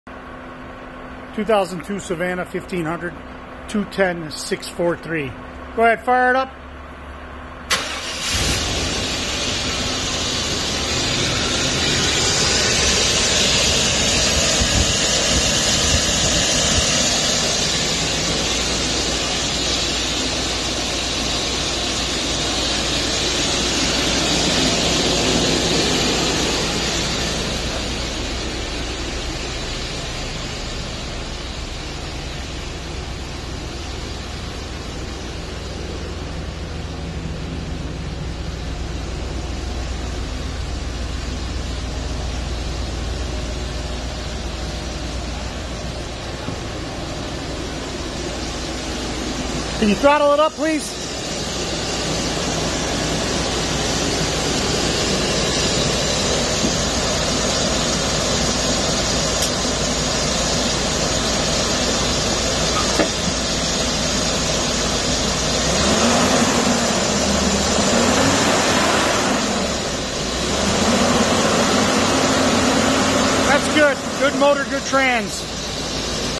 210643 2002 Savanna1500
vehicle testing @ Glen's Car and Truck

16000 Telegraph road Taylor Michigan